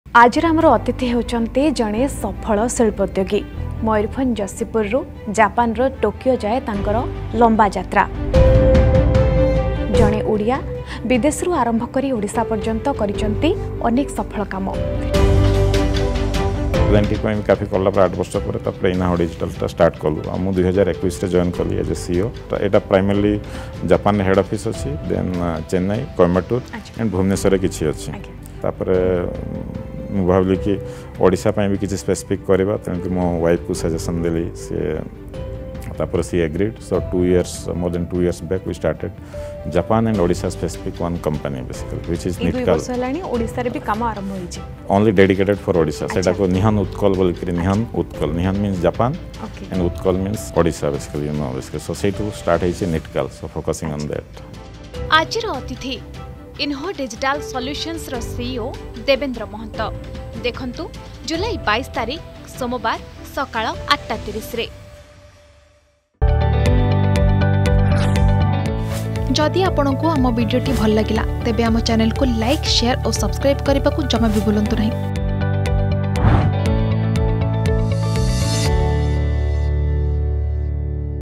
ଆଜିର ଅତିଥିରେ Inaho Digital Solutionsର ସିଇଓ ଦେବେନ୍ଦ୍ର ମହାନ୍ତ
ଦେଖନ୍ତୁ,ଜୁଲାଇ ୨୨ ସକାଳ ୮.୩୦ ରେ